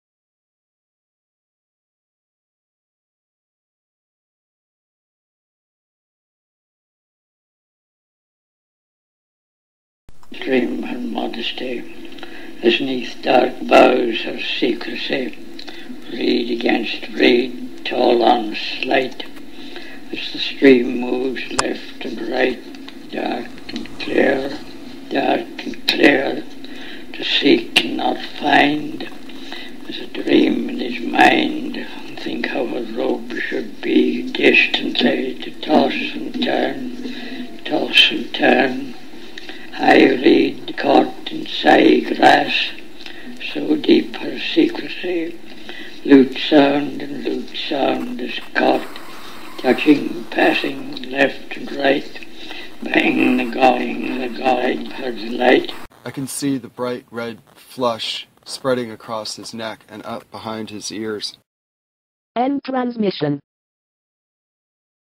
DICK 04
Note Ezra Pound reading from Confusian Odes in this one of 365 transmissions (22.Nov.12 - 22.Nov.13) of DICK, a book by Sam Truitt. For more search "DICK: An Oblique Kennedy Conspiracy Countdown."